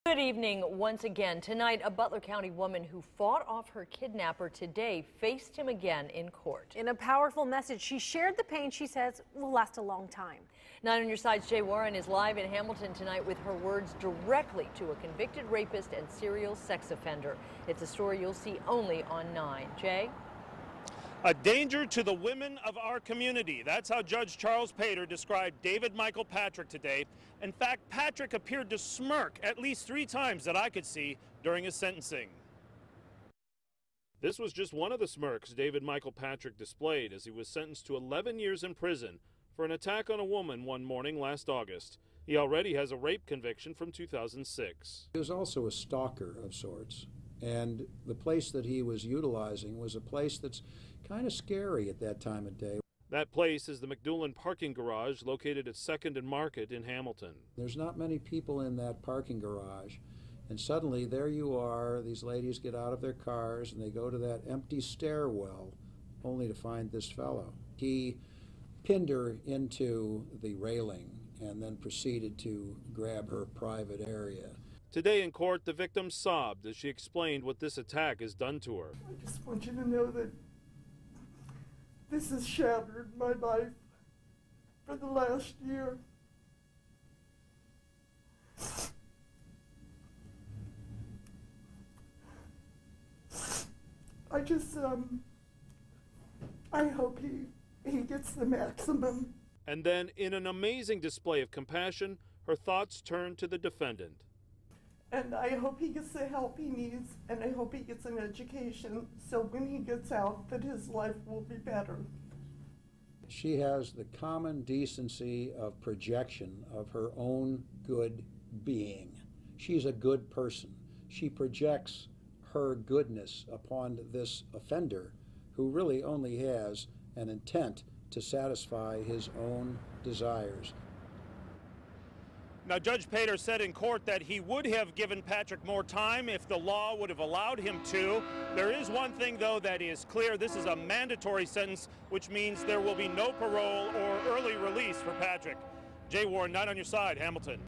Kidnapping victim speaks to abductor
David Michael Patrick sentenced Tuesday in Hamilton